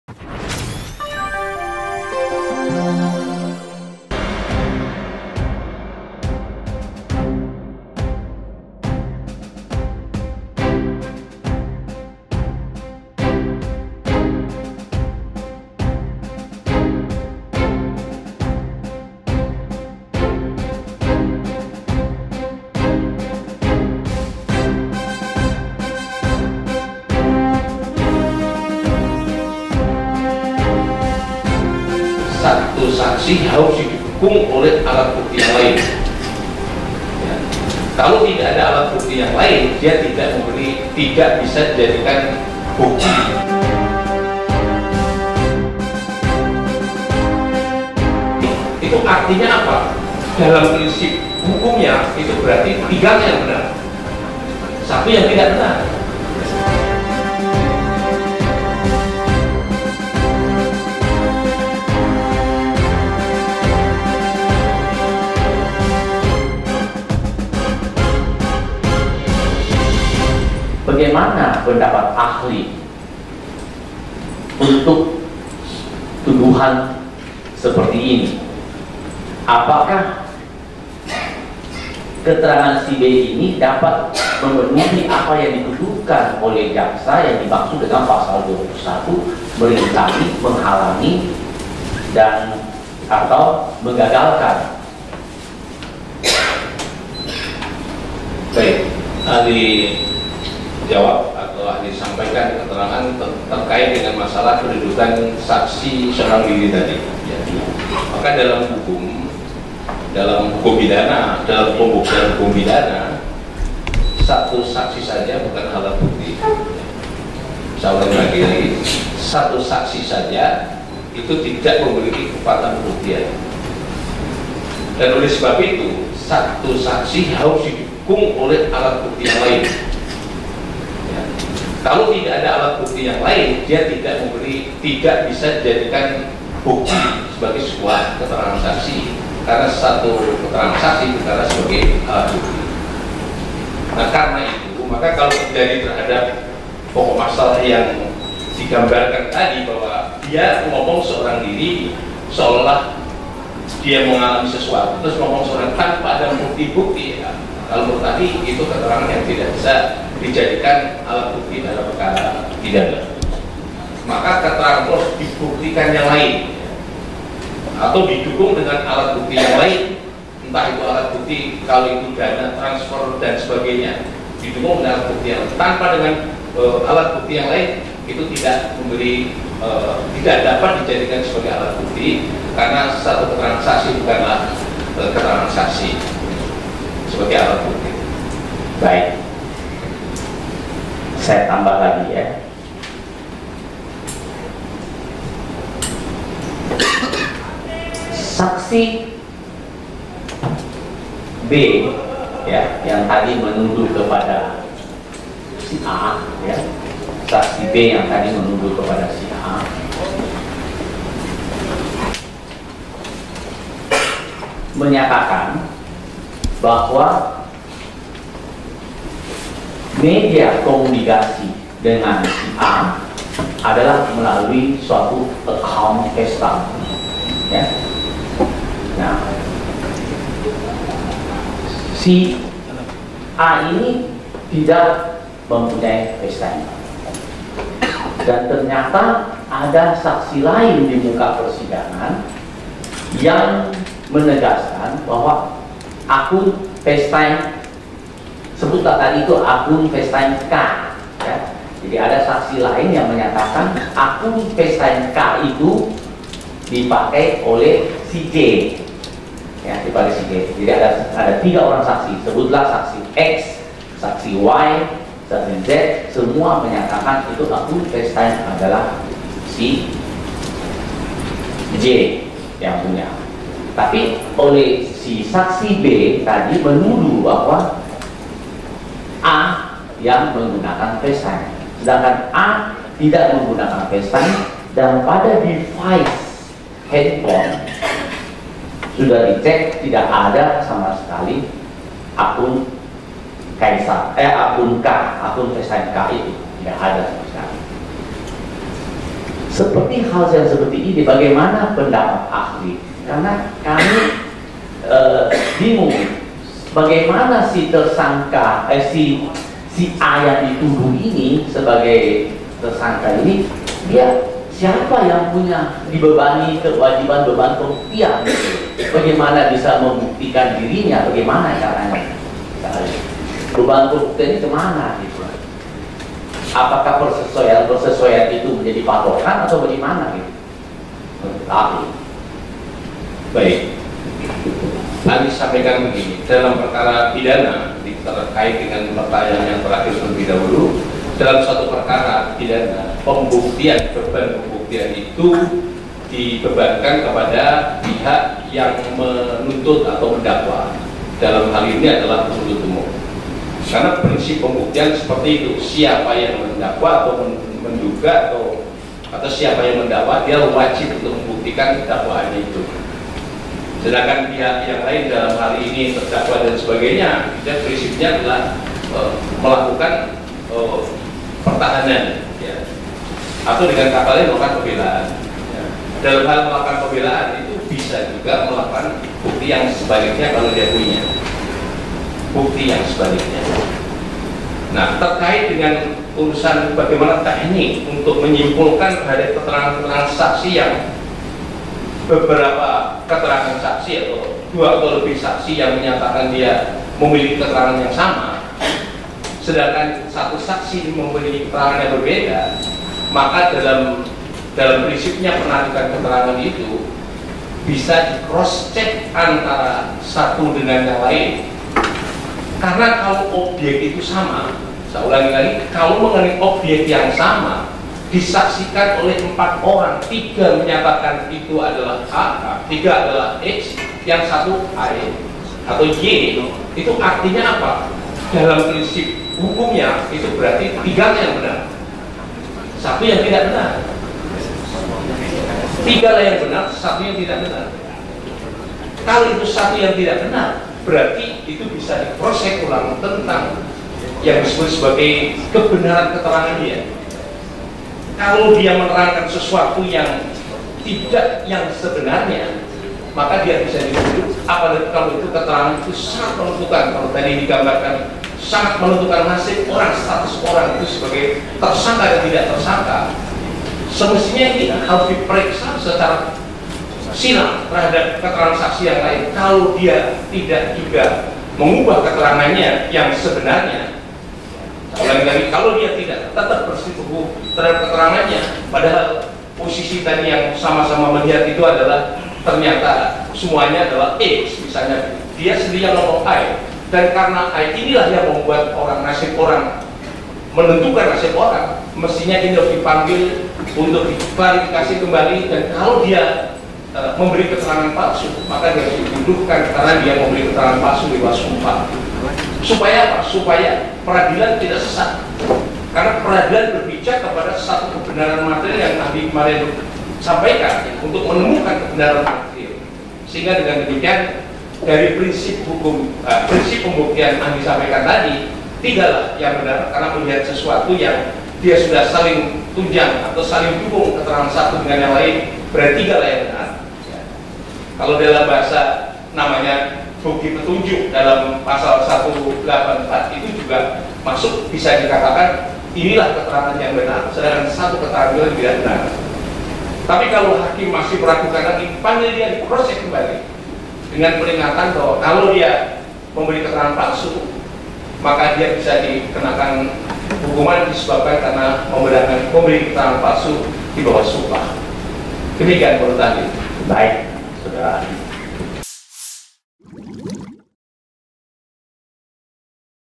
Saksi Ahli: Keterangan 1 Saksi Bukan Alat Bukti
SULSELSATU.com, JAKARTA - Sidang perkara perintangan penyidikan dengan terdakwa Lucas yang digelar di Pengadilan Negeri Jakarta Pusat, Kamis (21/2/2019) lalu menghadirkan ahli hukum dan pidana UII, Muzakkir. Keterangan saksi ahli dalam sidang kali ini membuat Jaksa Penuntut Umum (JPU) Komisi Pemberantasan Korupsi (KPK) kian terdesak.

Muzakkir membeberkan, keterangan satu-satunya saksi KPK dinilai lemah jika digunakan di persidangan. Hal ini disampaikan Muzakkir untuk menjawab pertanyaan Lucas terkait apakah keterangan satu saksi bisa digunakan untuk mendakwa seseorang telah melakukan perintangan terhadap penyidikan.

Menurut dia, dalam pembuktian hukum pidana satu saksi saja bukan alat bukti.

"Oleh sebab itu satu saksi harus didukung oleh alat bukti yang lain. Kalau tidak ada bukti yang lain dia tidak bisa dijadikan sebagai sebuah alat bukti," katanya.

Muzakkir menegaskan, keterangan saksi itu harus dibuktikan dengan alat bukti yang lain. Atau didukung dengan alat bukti yang lain.

Pembuktian dibebankan pada yang menuntut atau dalam hal ini penuntut. Penuntut, dalam hal ini jaksa, wajib membuktikan tuntutannya itu.

Jika berbeda bisa dikroscek satu dengan yang lain.

Dalam hal ini satu saksi pun tidak terpenuhi , karena Saluang berbantahan dan tidak ada persesuaian dengan saksi lain. Ada 3 saksi di bawah sumpah dimuka persidangan, yang tidak sesuai dengan keterangan Dina Soraya ( yaitu SS, MS dan ES) . Di samping itu, Dina sendiri keterangannya inkonsisten dengan keterangan sendiri pada BAP di kasus ES

"Jika ada empat saksi dimana tiga di antaranya sama dan satunya berbeda, maka secara hukum yang tigalah yang benar. Yang satu bisa dikroscek kebenaran kesaksiannya. Diperiksa secara silang. Kalau tiba-tiba dia merubah keterangannya, maka keterangannya perlu dikroscek ulang," tegas Muzakkir.

Jika keterangan satu saksi berbeda dengan saksi yang lain yang ikut menyaksikan tindak pidana, kata Muzakkir, mestinya dia dipanggil untuk diklarifikasi kembali.

"Kalau dia memberi keterangan palsu, maka dia harus dituduhkan memberi keterangan palsu di bawah sumpah. Supaya peradilan tidak sesat, karena peradilan berpijak pada satu kebenaran materiil," ujar dia.